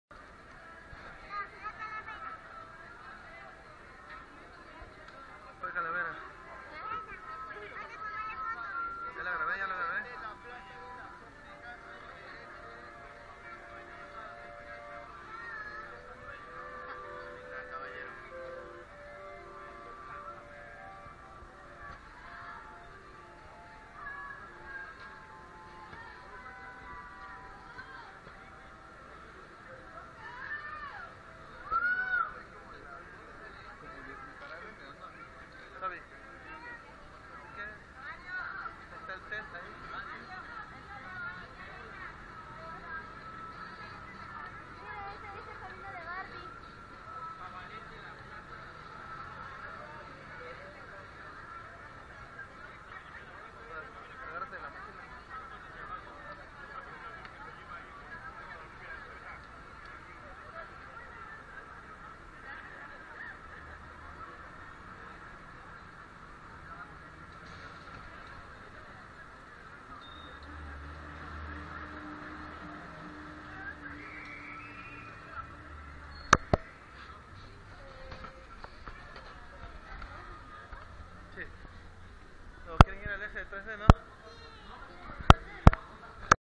Calle Francisco I. Madero rumbo a la Torre Latinoamericana
Caminando en la calle Francisco I. Madero - Bellas Artes - Torre Latinoamericana